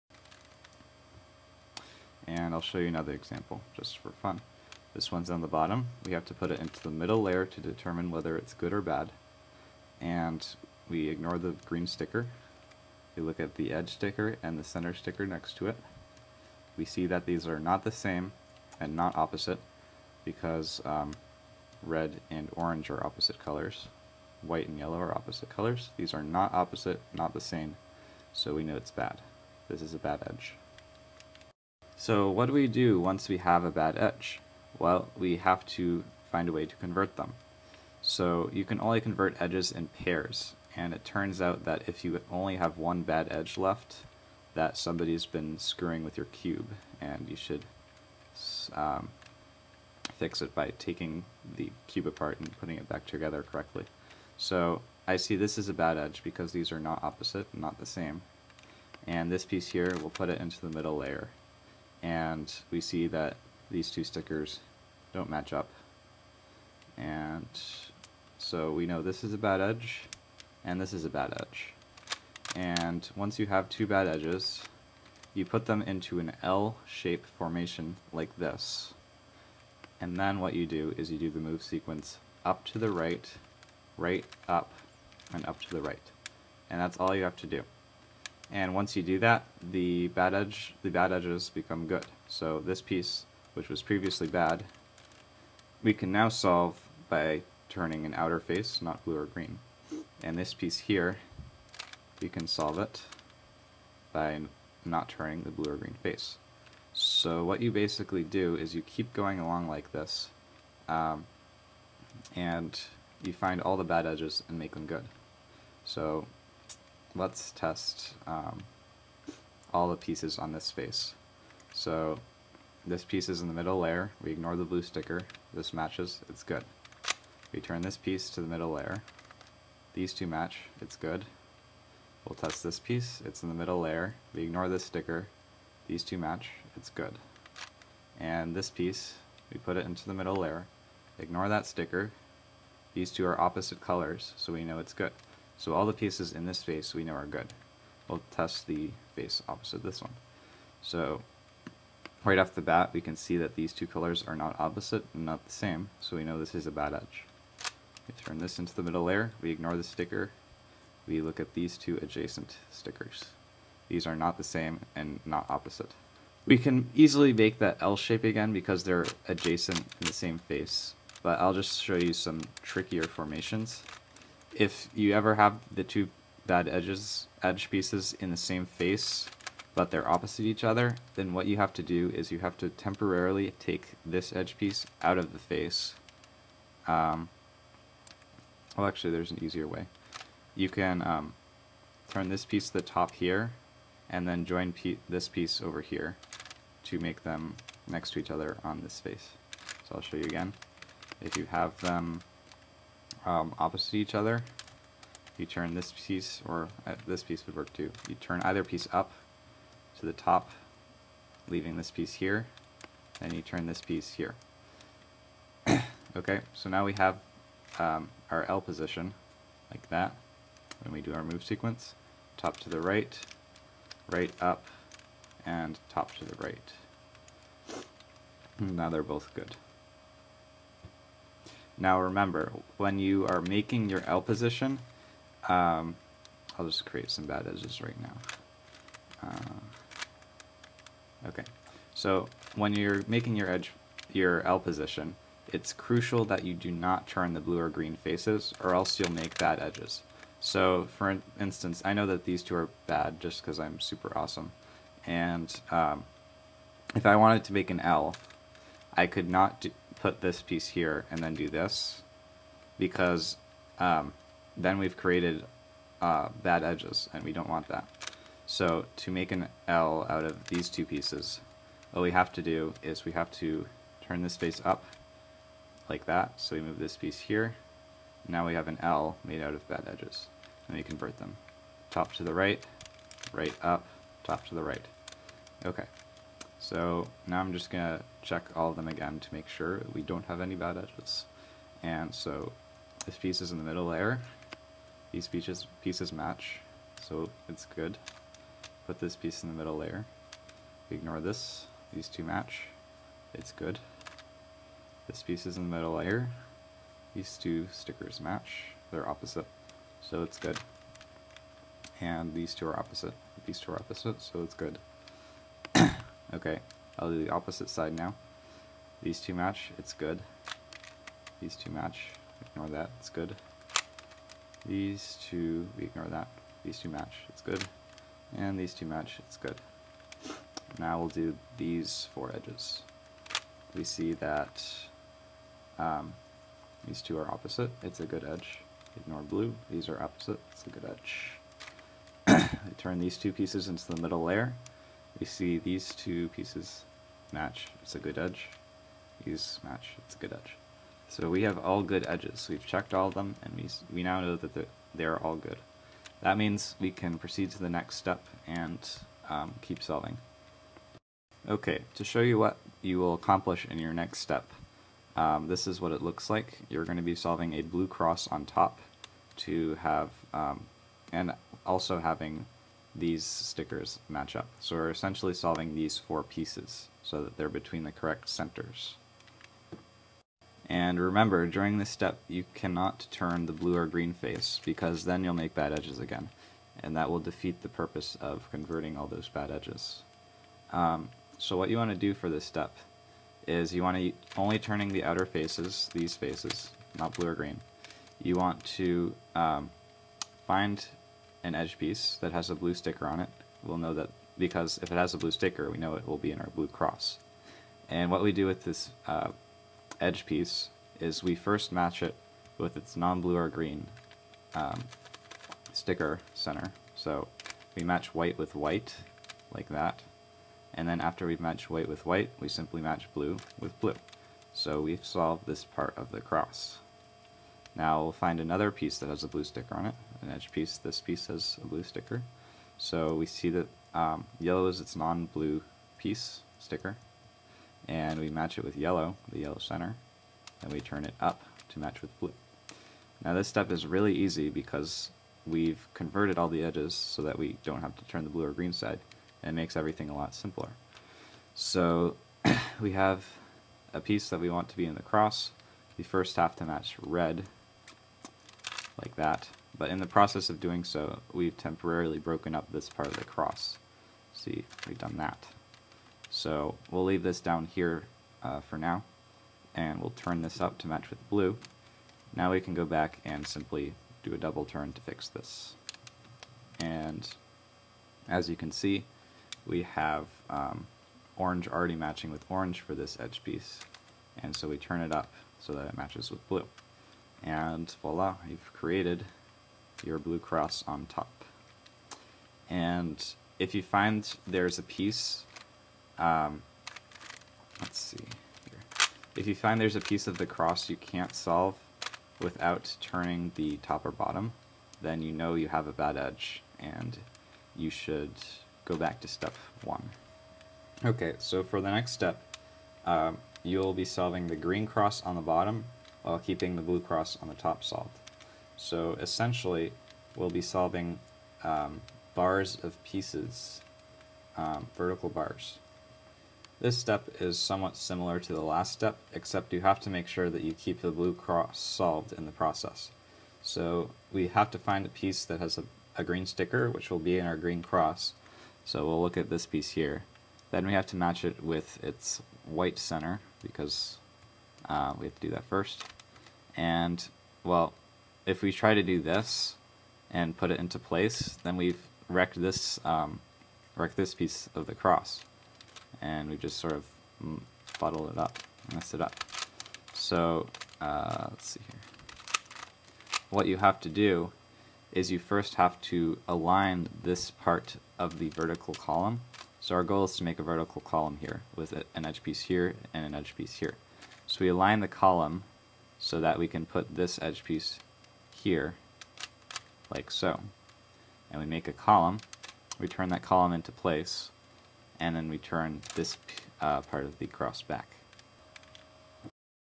Rubik's Cube for Beginners (Edges First; Part 2)
This is a beginner's guide for how to solve the Rubik's cube. No prior knowledge of the cube is necessary. If you get stuck on a step, watch the video step again and read the guide below. If you're still stuck, please ask me for help!

STEP 1: Fix the orientations of all edges (make them all "good").

To find out if an edge is "bad" or "good":
1. If the edge is not in the middle layer, turn the face containing it (NOT the blue or green face) to place the edge in the middle layer.
2. IGNORE the blue/green sticker on the edge piece (if one exists).
3. Look at the centre next to the non-blue/green sticker on the edge. If the edge sticker and centre are the same or opposite color, the edge is GOOD; otherwise, it is BAD.

To fix 2 bad edges on the front face (one on top, one on the side in an "L" shape), do UR RU UR. Fix all bad edges which you can find until there are none left. Remember, do not turn the blue or green side unless you are converting edges!

STEP 2: Solve a blue cross on top.

Find an edge piece with a blue sticker, match it with one of its centres, then match it with the blue centre. Repeat until the cross is done. Remember not to turn the top or bottom sides (blue/green)!

STEP 3: Solve a green cross on the bottom.

Find an edge with a green sticker, and place it on the opposite side of its respective blue cross edge, forming a "line". Turn the line so the blue sticker is on top again, and fix any other lines which you turned. Repeat.

STEP 4: Solve the remaining edges.

Count how many are incorrectly positioned, and see which case you have below:

4 unsolved edges-
double turn any 2 adjacent faces (EXCEPT blue/green) until at least one edge becomes solved.

2 unsolved edges (not same face) or 3 unsolved edges-
Double turn two adjacent faces which contain all of the unsolved edges.

2 unsolved edges (same face)-
If the unsolved edge pieces are in the SAME face, hold the edges so both are in the front face, then repeat RD F2 until they become solved.

STEP 5: Solve the top 4 corners.

1. Find a corner on the bottom with a blue sticker. Find its appropriate spot on top, and rotate the bottom so that the corner is under that spot. Hold the corner and slot on the right side, and do:
RU  UL  LU  UR
RD  UL  LD  UR
This algorithm cycles the bottom-left corner, bottom-right corner, and top-right corner in a counterclockwise fashion. Keep positioning top layer corners until they are all in place.

2. Orient all top layer corners. You use this algorithm to flip the top-front corners:
BR  RU  BL  RD
FR
RU  BR  RD  BL
FL
This algorithm flips the right corner clockwise and the left one counterclockwise. Orient the cube in a way that will flip corners correctly!

STEP 6: Solve the remaining corners.

1. Turn the bottom layer so that all the edges are correctly positioned again (if they aren't already).

2. Cycle the corners into place using the algorithm from step 5.

3. Orient the corners, like in step 5.